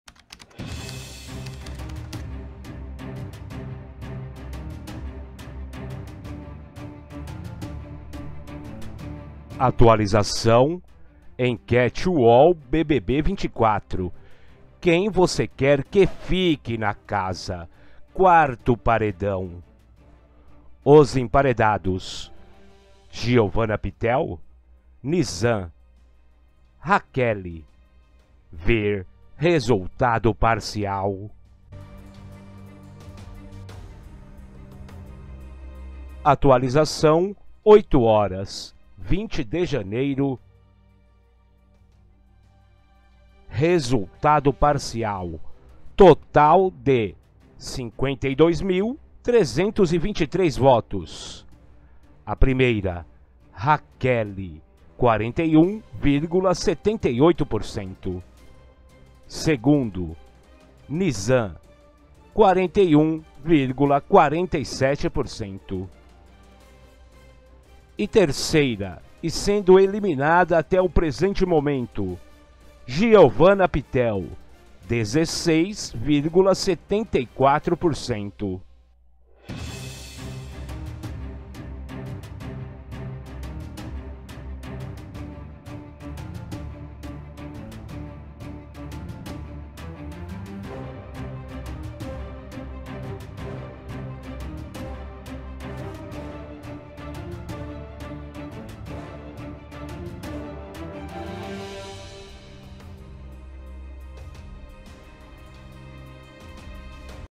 ENQUETE UOL BBB 24 atualizado 8h dia 20 '4º paredão' EMPATE DUPLO
BBB 24 - enquete UOL: Quem você quer que fique na casa? Giovanna Pitel, Nizam e Raquele
52.323 votos
Resultado parcial
1
Raquele
Raquele

41,78%
2
Nizam
Nizam

41,47%
3
Giovanna Pitel
Giovanna Pitel

16,74%
A votação do UOL não possui caráter científico ou influência sobre o resultado do programa da TV Globo